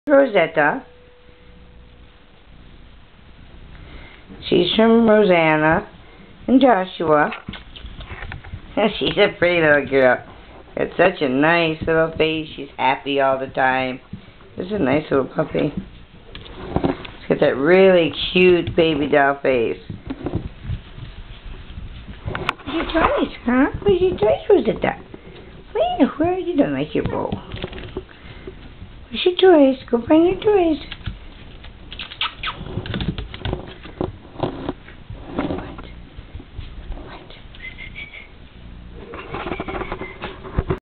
Little Yorkie Rosetta
Rosetta
D.O.B. - 8/24/09
weight - 1-lb 10oz
This is a beautiful little puppy she has a nice happy go lucky personality and she loves to play. She is AKC reg. and warrantied. to see my other puppies please visit my